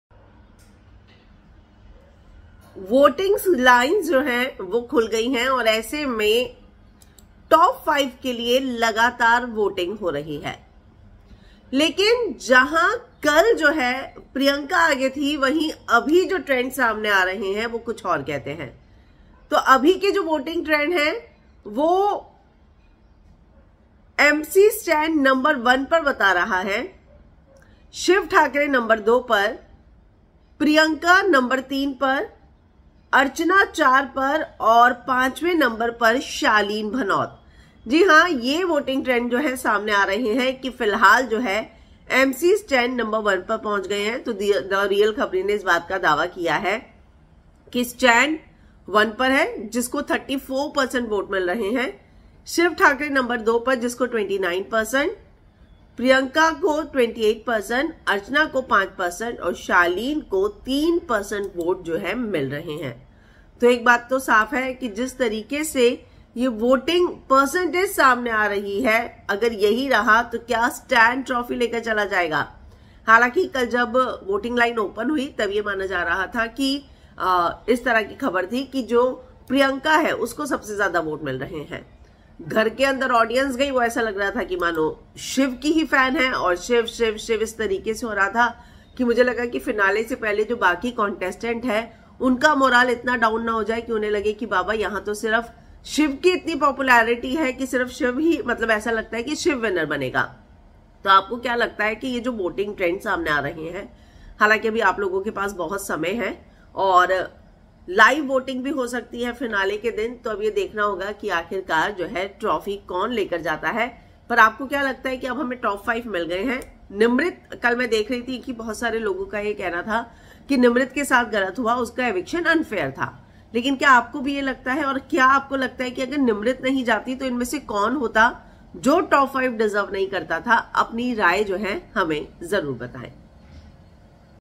Bigg Boss 16 Kya Kehte hai Voting Trends, Priyanka-Shiv ko laga bada jhatka
Bigg Boss 16 Nimrit Eviction Interview Priyanka ne Kyo aate

Video Rights: TV Window
Anchor: Heena Kumawat